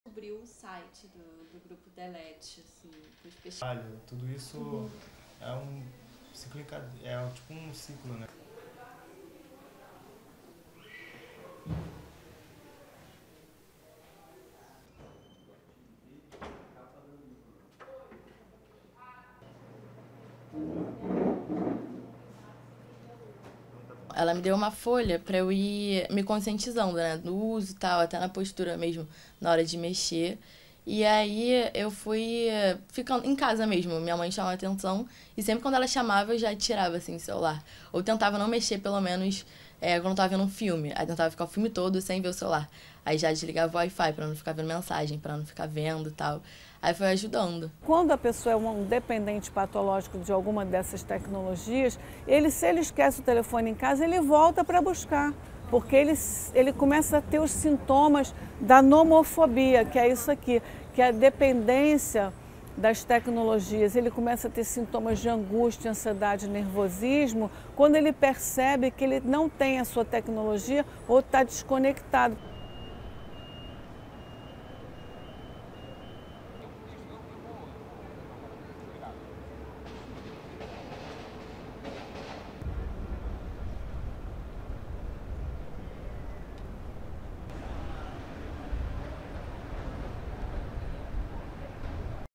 Nomophobie: la dépendance au portable soignée à Rio
Ils possèdent plusieurs téléphones portables, angoissent s'ils n'ont plus de batterie ou de réseau. Des personnes atteintes de "nomophobie" (de l'anglais "no mobile phobia", la phobie d'être séparé de son téléphone) sont prises en charge à Rio de Janeiro.